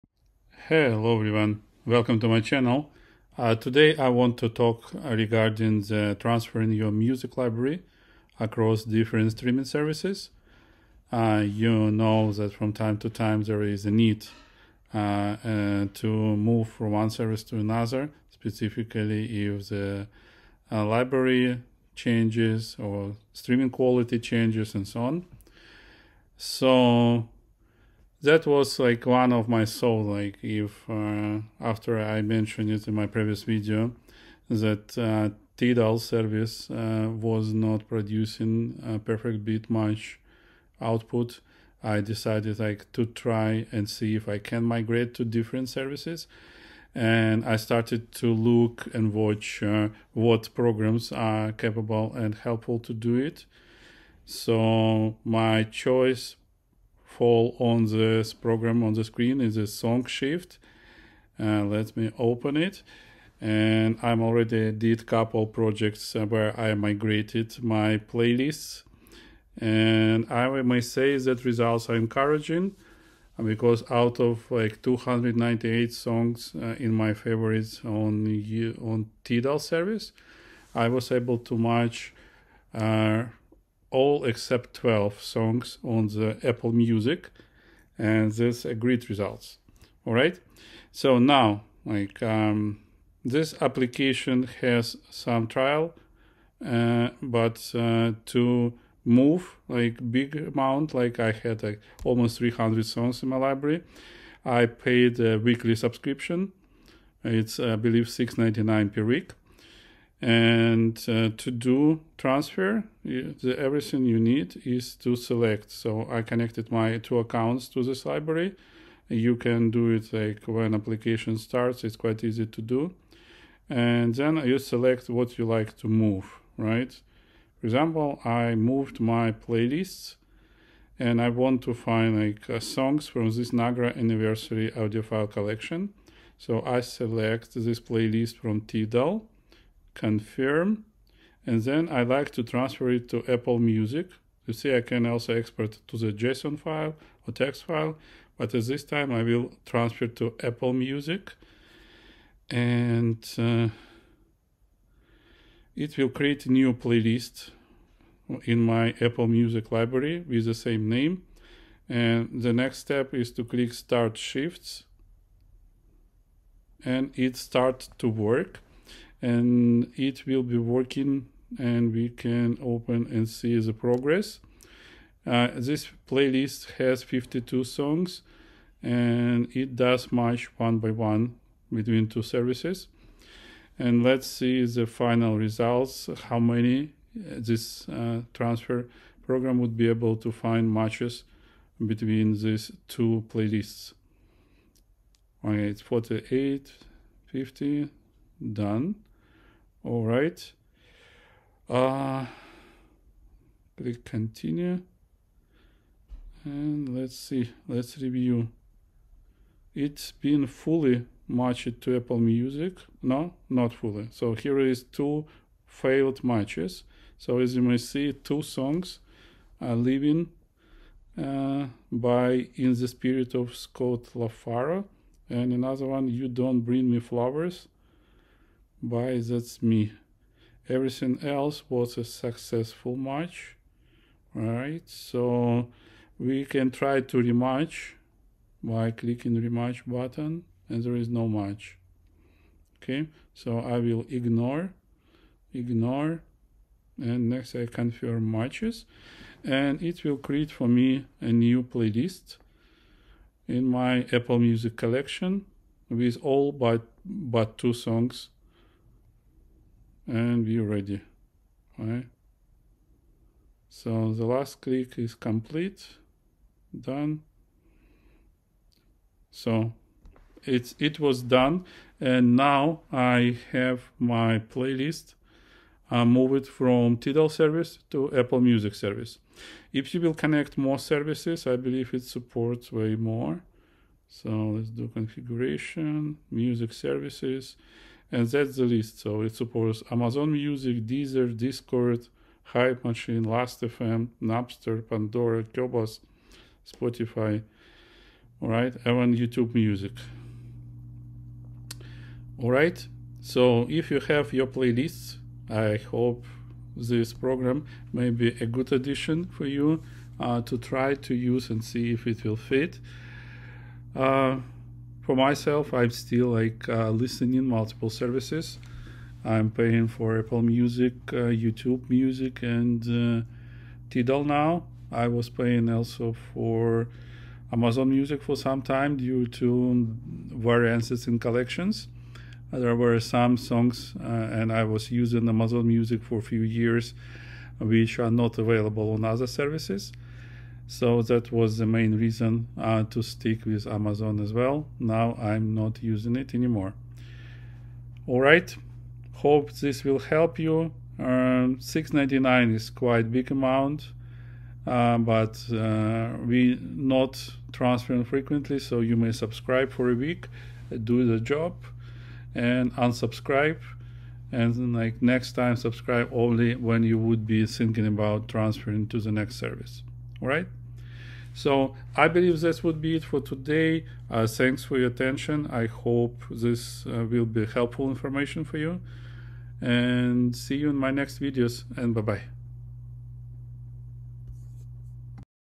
How to Transfer Playlists Between Streaming Services: SongShift Review
Switching between music streaming services can feel overwhelming, especially when it comes to transferring your carefully curated playlists, albums, and library. Many assume it’s an impossible task, but with SongShift, the process becomes surprisingly easy.

In this video, I share my experience using SongShift to migrate my playlists between different services. Whether you're moving from Tidal to Apple Music, Spotify to Qobuz, or any other combination, this app simplifies the process while keeping your music organized.

🔍 Key Features of SongShift:

Seamless Playlist Transfer: Easily move your favorite playlists, albums, and saved songs from one streaming service to another.
Fast & Efficient Matching: SongShift automatically finds and matches tracks across platforms.
Customization & Manual Adjustments: If a track isn’t found, you can manually select the best match to keep your playlist accurate.
Supported Services: Works with Apple Music, Spotify, Tidal, Qobuz, YouTube Music, Deezer, and more.
📖 Why It’s Useful:

No More Rebuilding Playlists from Scratch: Save time and frustration when switching to a new streaming service.
Keeps Your Library Intact: Ensures your favorite tracks and albums move with you.
Great for Exploring Alternatives: If you're dissatisfied with your current service, this makes switching easier.
👂 Share Your Experience:

Join the Conversation: Have you used SongShift or any other playlist transfer tool? Share your thoughts, tips, or alternative apps in the comments below!
If you're considering switching streaming services, this guide will help you migrate your music library hassle-free.